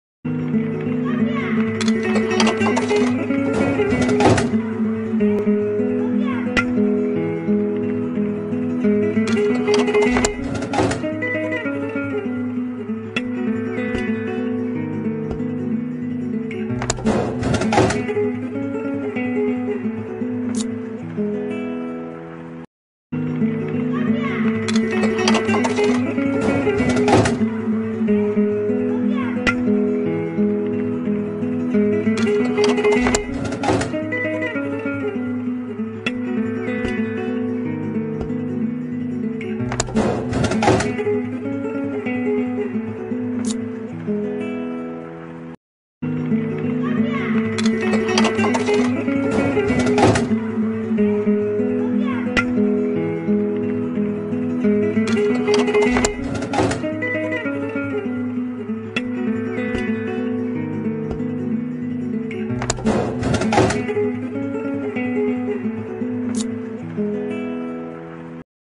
Рекламани ёмони 😳
Айтишларича Шу реклама учун Pepsi компанияси даҳшатли жарима тўлаган:)